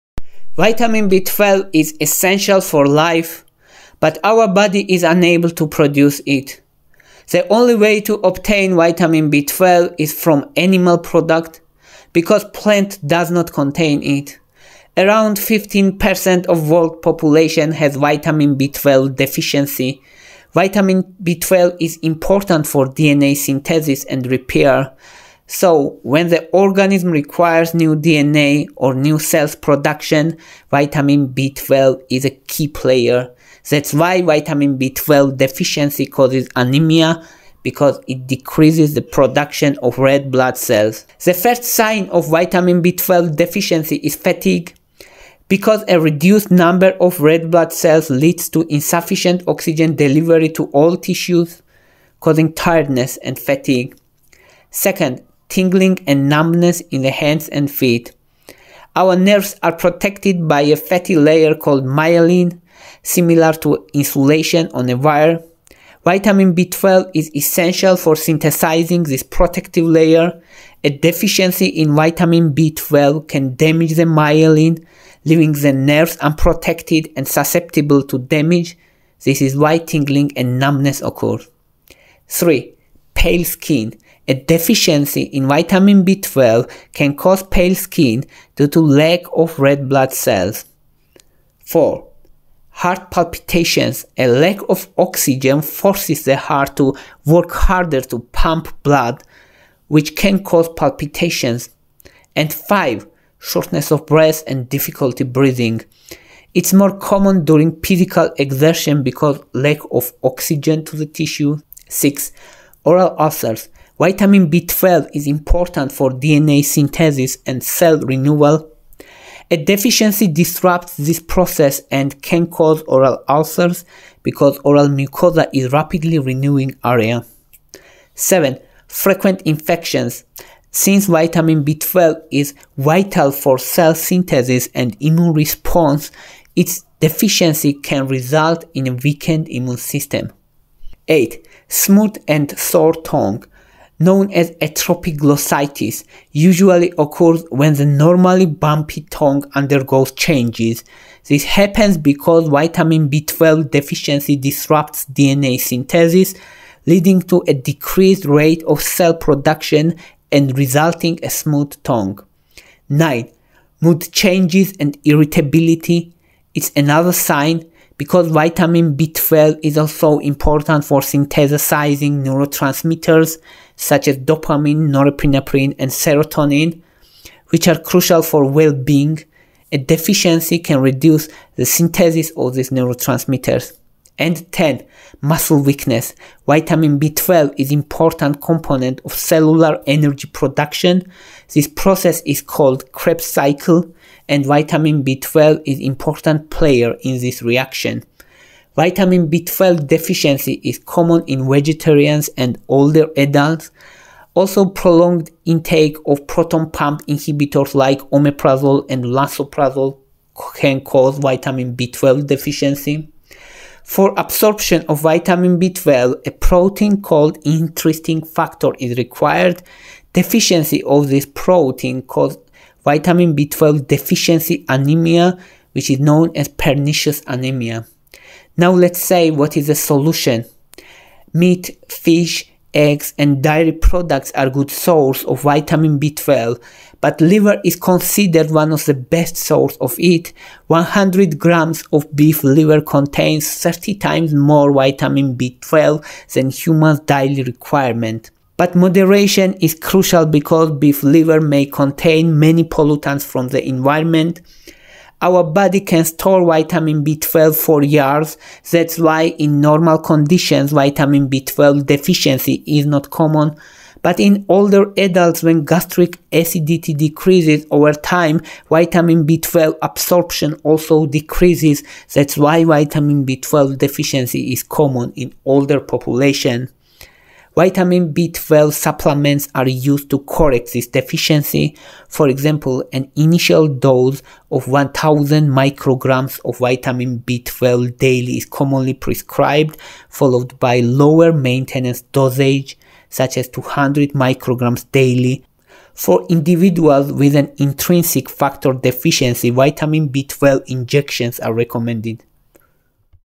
Are You B12 Deficient? 10 Symptoms You Shouldn't Ignore
Essential Nutrient: Vitamin B12 is crucial for life, necessary for DNA synthesis, red blood cell formation, and neurological function.
Sources of B12: Since the human body cannot produce Vitamin B12, it must be obtained from animal products such as meat, fish, eggs, and dairy, as plants do not contain this vitamin.
Deficiency Symptoms: Symptoms of Vitamin B12 deficiency include weakness and fatigue, tingling and numbness in fingers and toes, pale skin, heart palpitations, shortness of breath, mouth ulcers, frequent infections, smooth and sore tongue, and mood changes.
Risk Groups: Vegetarians, older adults, and individuals taking certain gastric medications (like proton pump inhibitors) or with pernicious anemia are at higher risk for Vitamin B12 deficiency.
Neurological and Cellular Impact: Vitamin B12 is essential for the synthesis of myelin, which protects nerves, and for the energy-producing Krebs cycle within cells.
Anemia and Blood Health: Deficiency leads to anemia due to its role in DNA synthesis and red blood cell formation.
Supplementation Needs: Higher doses of Vitamin B12 are often used in maintenance therapy to compensate for absorption inefficiencies and to buffer against deficiencies.
Storage and Safety: Vitamin B12 is stored in the liver and is safe at higher doses due to its water-soluble nature, allowing excess to be excreted.
Therapeutic Doses: In cases of severe deficiency or conditions like pernicious anemia, high therapeutic doses of Vitamin B12 may be administered, sometimes via injection.
Dietary Considerations: Regular consumption of Vitamin B12-rich foods like beef liver is advised, but moderation is necessary due to potential contaminants in liver products.